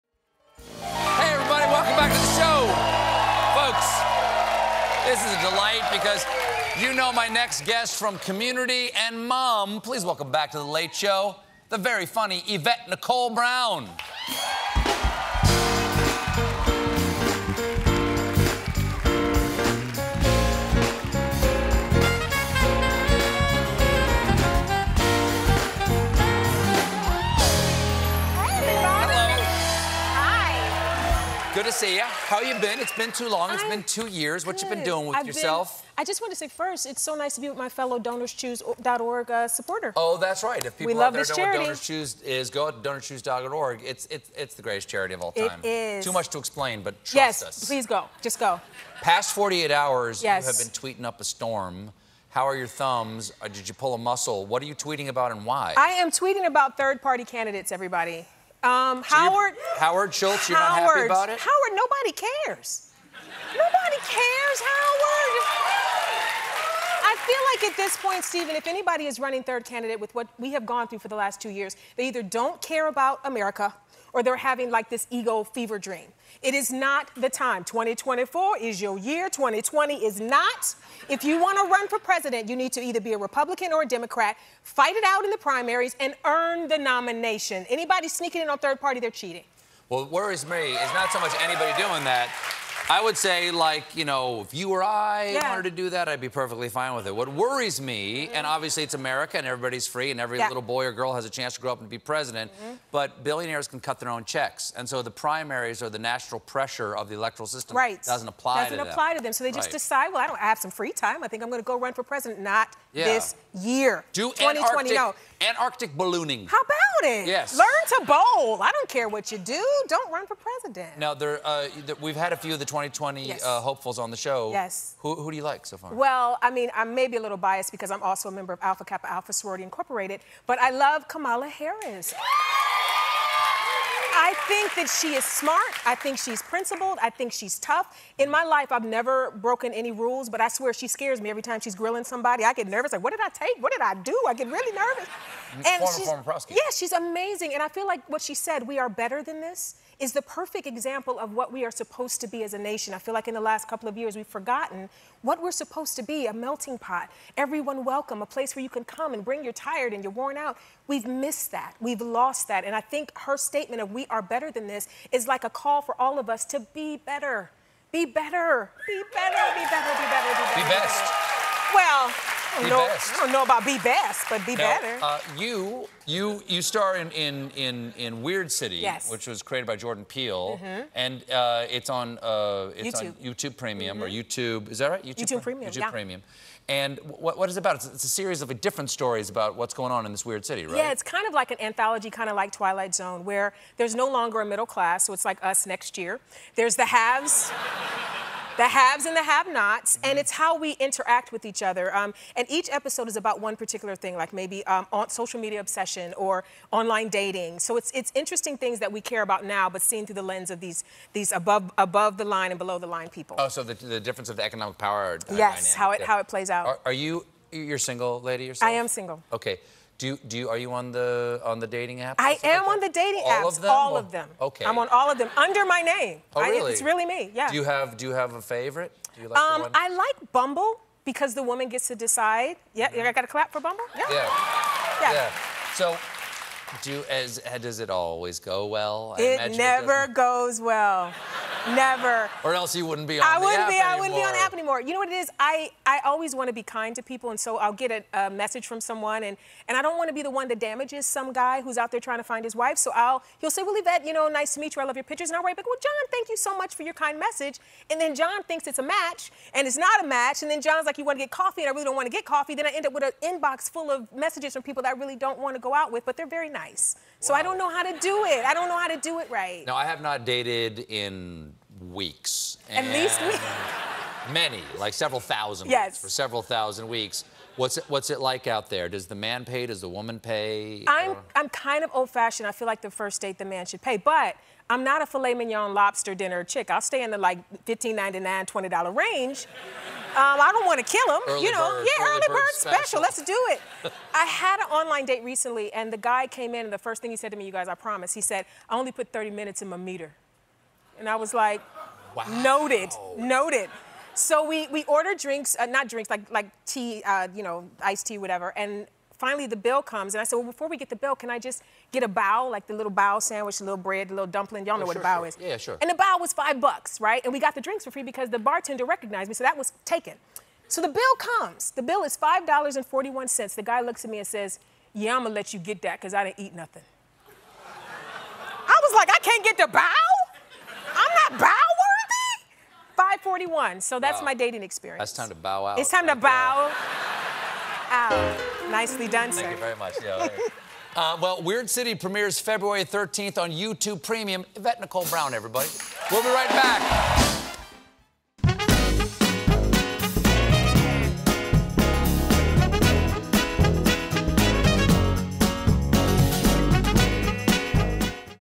Yvette Nicole Brown: No Third-Party Candidates!
'Weird City' star Yvette Nicole Brown has been tweeting up a storm about Howard Schultz.